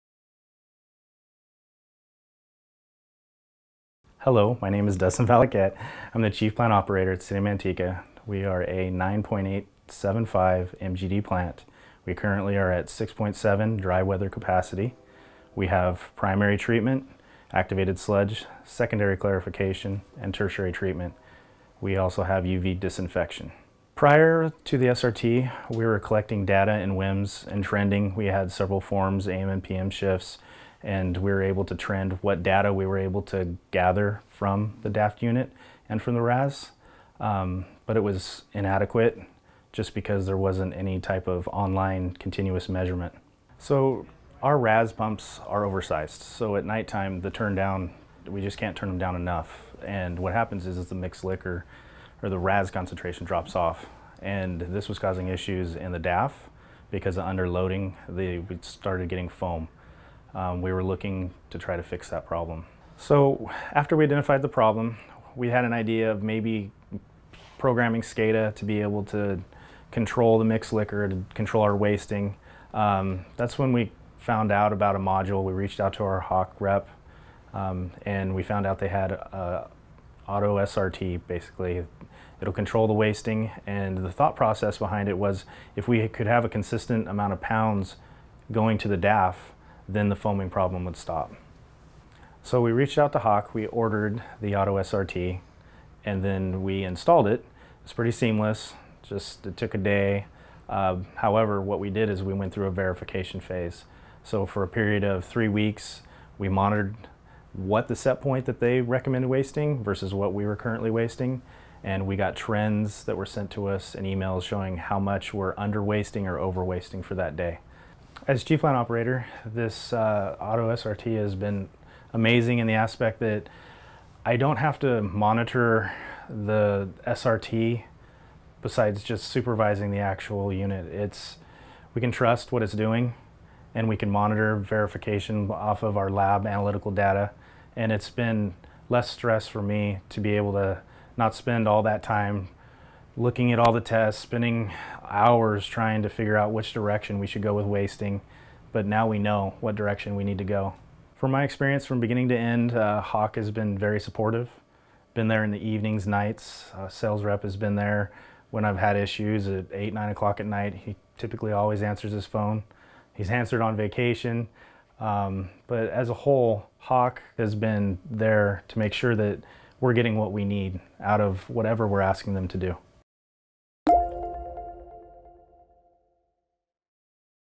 Optimizing Sludge Retention Time (SRT) in Wastewater in Manteca, CA with Claros
Listen to the Chief Plant Officer at the City of Manteca, Waste Water Quality Control Facility in California as he discusses the optimization of sludge retention time (SRT), and an issue excess foaming at night with their Dissolved Air Floatation Thickener (DAF) - and how Claros, the Water Intelligence System from Hach was their solution.

Specifically, Manteca needed the following and Claros, the Water Intelligence System from Hach was their solution:
- 24/7 visibility into their RAS/WAS concentration
- The ability to adjust the process accordingly to prevent the DAF foaming
- Creation of a repeatable and precise SRT
- Visual management tools, alerts and notifications to make real-time decisions
- Complete insight into all data - anytime, anywhere